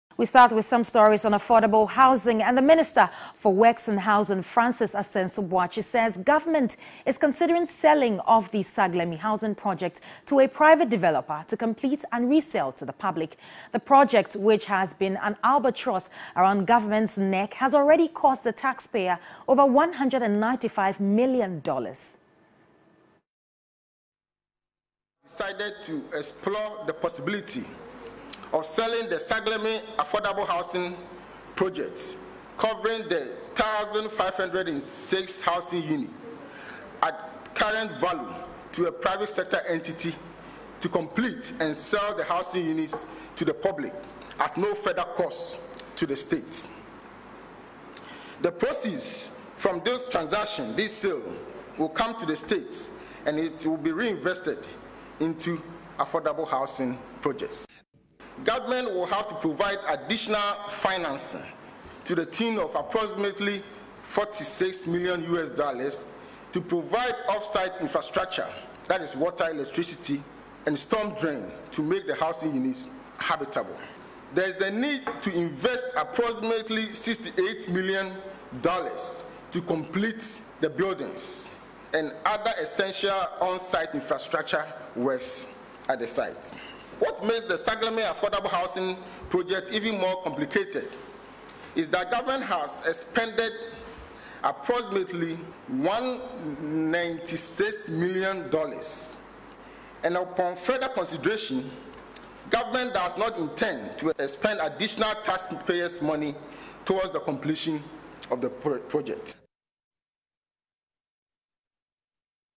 Government to sell Saglemi housing project to private developer.
According to the Minster of Works and Housing, Francis Asenso Boakye, the funds that will be generated from the sale of the Saglemi housing project will be reinvested into another housing project.